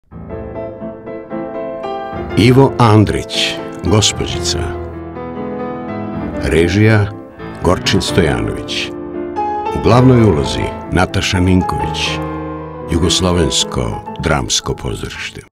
GOSPOĐICA
Ivo Andrić
Reditelj Gorčin Stojanović

Naslovnu ulogu igra Nataša Ninković a u ostalim ulogama su: Jasmina Avramović, Vojislav Brajović, Srđan Timarov, Nataša Tapušković, Marko Baćović, Vesna Stanković, Anđelika Simić, Milan Marić, Stefan Bundalo, Dejan Dedić.

Dramaturg je Miloš Krečković, kostimograf Lana Cvijanović a pored režije, Gorčin Stojanović potpisuje i scenografiju. Dizajner zvuka je Robert Klajn.